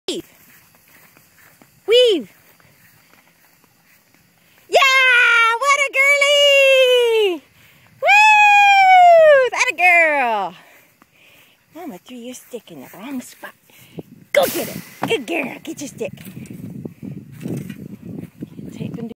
Gilley Weaving 12 Poles 8/9/10
So, I accidentally threw her stick outside the agility field fence when we were practicing and had to retrieve. I got a little bold & asked her to weave on our "hard" side with me still outside the fence - I haven't asked her with me at this distance away from her. Needless to say, she was a rock star! Slower than normal, but no skipped poles! Wahooo! We ran straight to the pond for reward since I can't throw that damn stick to save my life :)